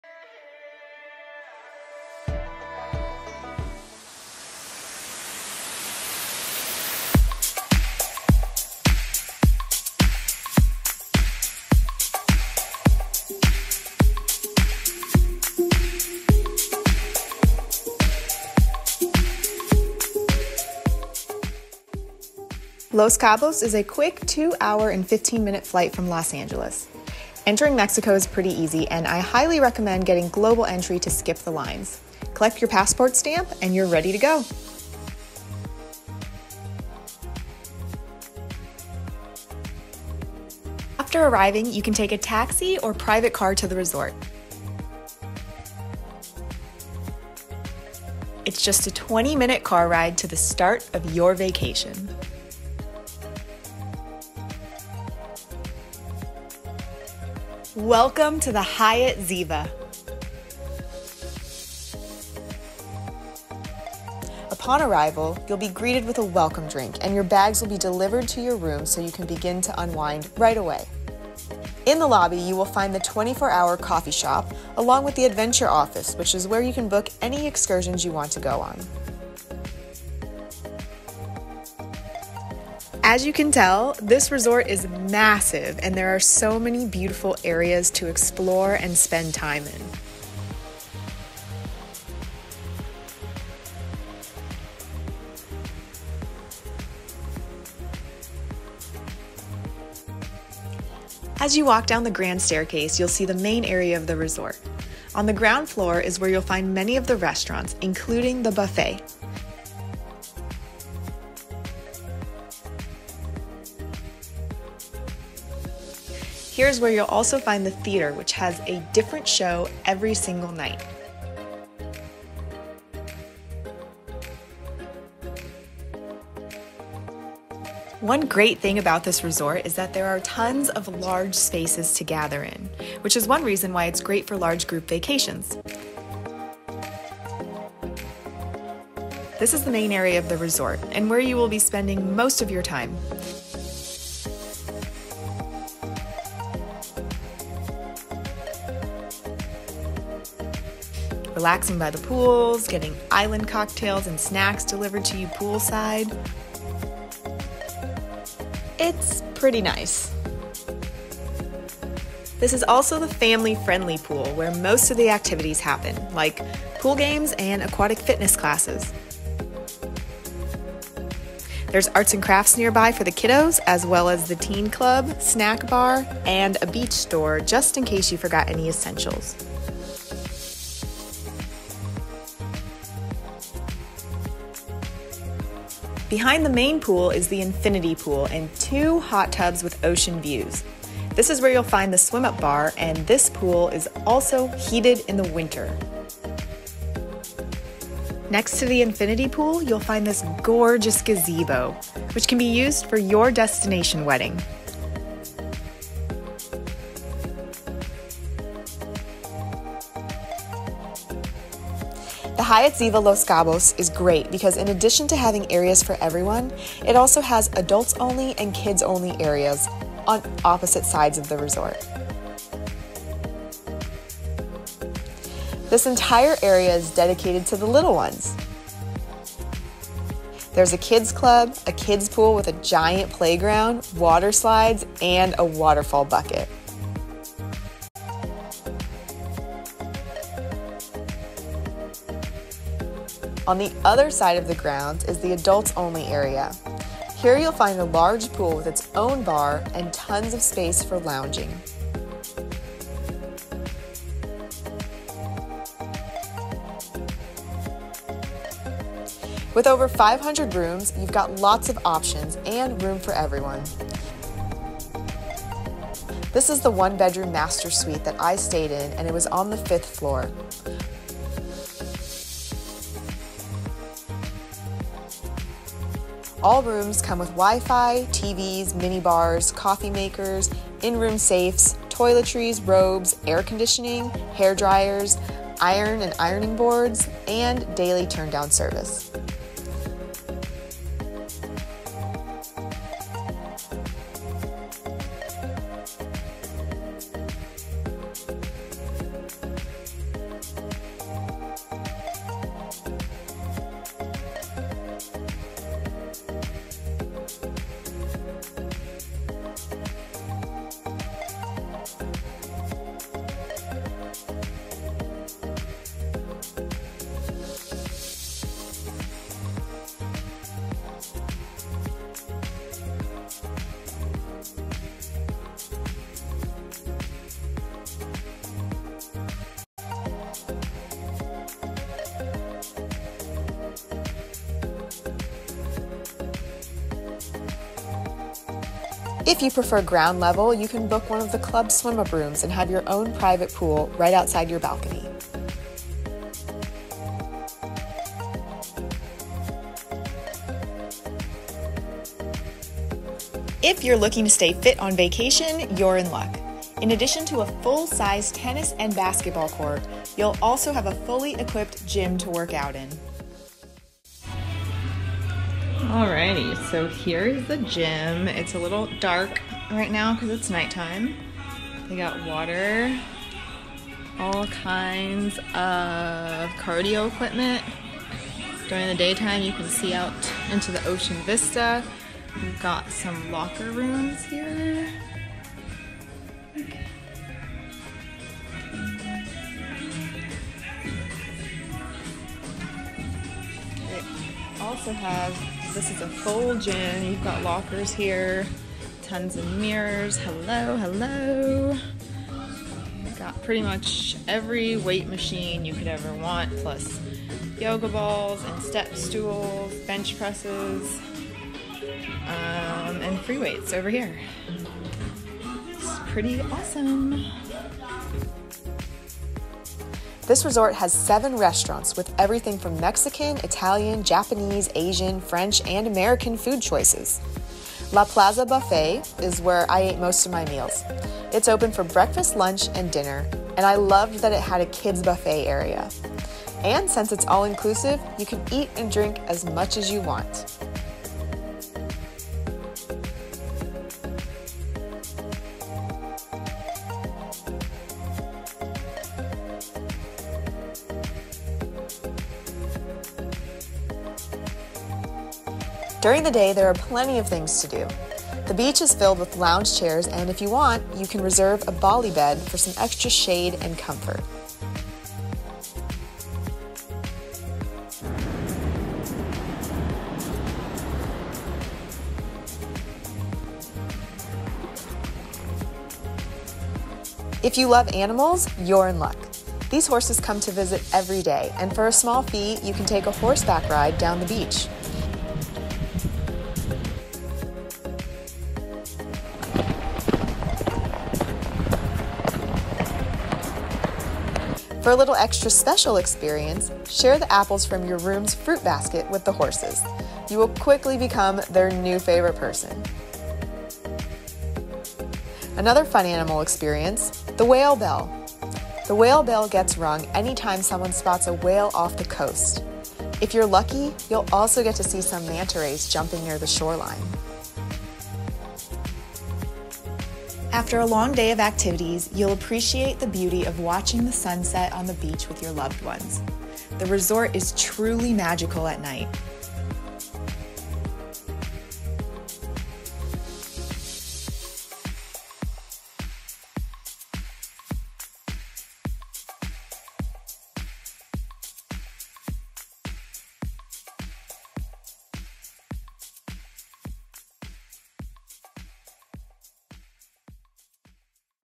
2022 All-Inclusive Hyatt Ziva Los Cabos | FULL TOUR | Mexico
Full Tour of the Hyatt Ziva Los Cabos in Cabo San Lucas, Mexico! (2022)

Welcome to the Hyatt Ziva Los Cabos! A World of Hyatt All-Inclusive, oceanfront resort located on the Baja California Peninsula. With 7 restaurants, 3 gigantic pools (including a kids-only and adults-only), spacious rooms, and miles of beautiful beach, this family-friendly resort is the perfect place for your girl's trip, family vacation, corporate convention, or romantic weekend getaway. There truly is something for everyone here!
Quick Reference:
0:22 -- Getting There
1:07 -- Resort Lobby
2:26 -- Pools & Main Areas
3:45 -- Kids Only Area
4:04 -- Adults Only Area
4:20 -- Rooms (One Bedroom Master Suite Tour)
5:51 -- Fitness Areas
6:59 -- Restaurants
7:34 -- The Beach
7:54 -- Animal Encounters
8:40 -- Sunsets

Have fun in Mexico!!
For more photos & BTS, find me online =)